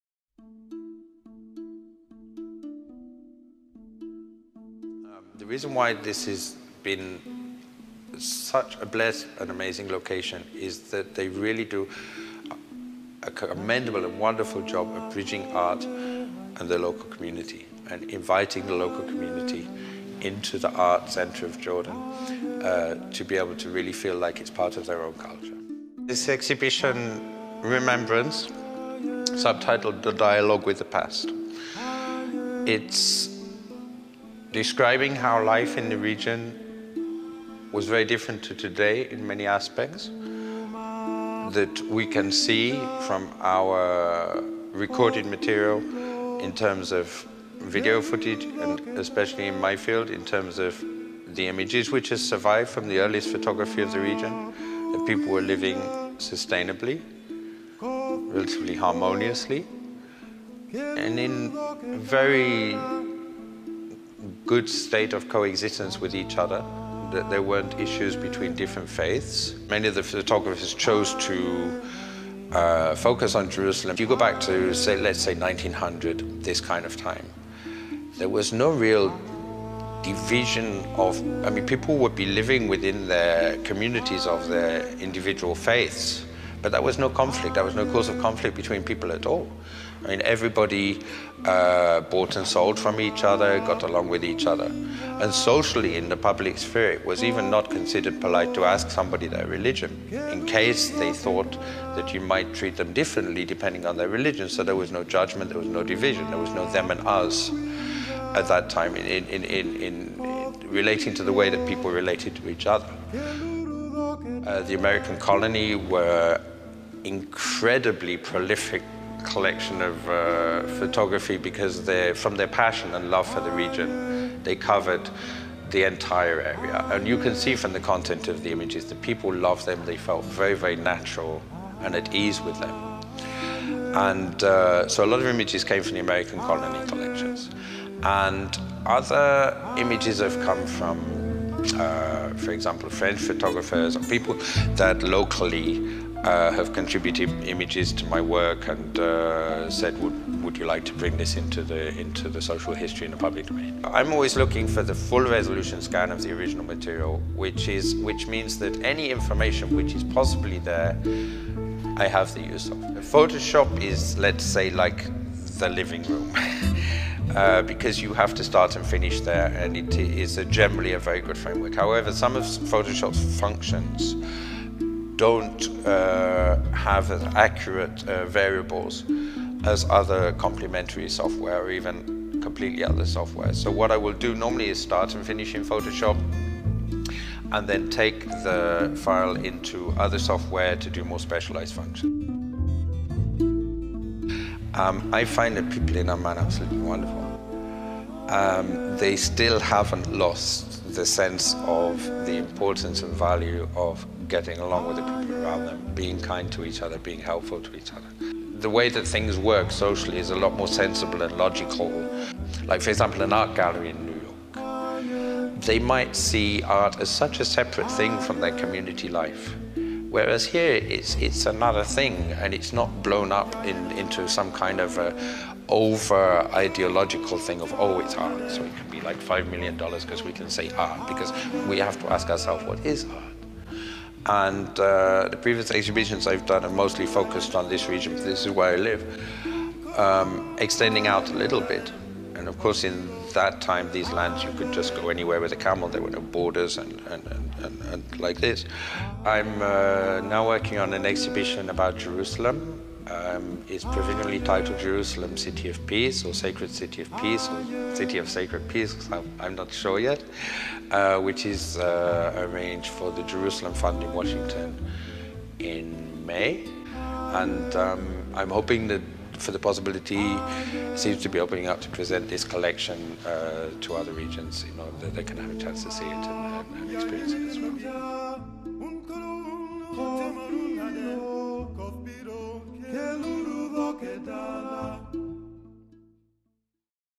Remembrance a Dialogue with the Past an exhibition by Kelvin Bown
Interview with Kelvin Bown at The Jordan National Gallery, January 2018.